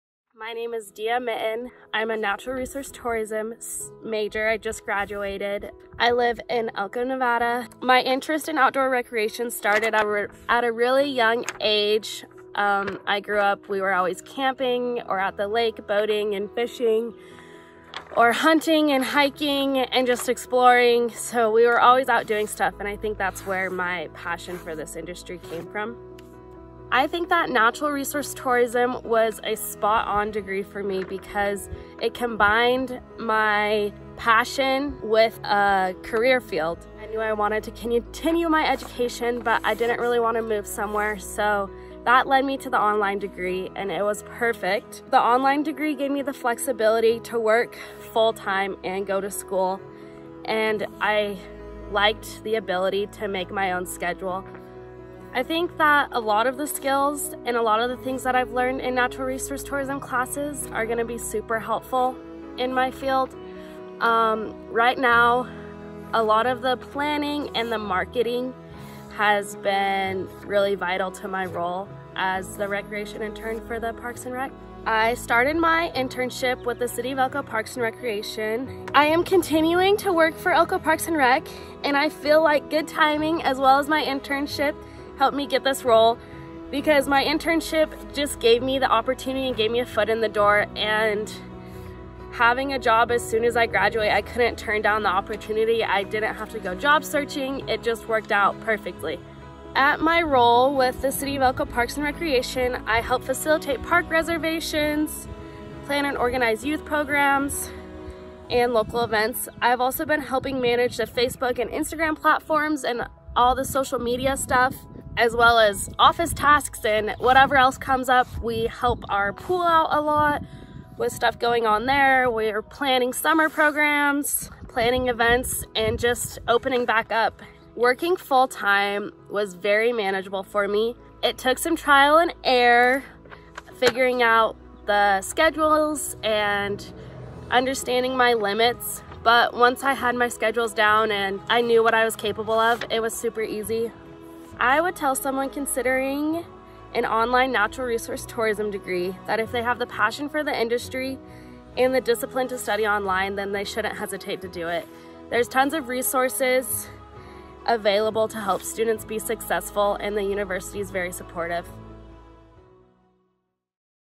Dea Mitton – Natural Resource Tourism Online Alumna
Dea Mitton of Elko, NV was our first alum of the online B.S. Natural Resource Tourism undergraduate program. Hear more about what drives her, why she chose NRT Online and her career post-graduation.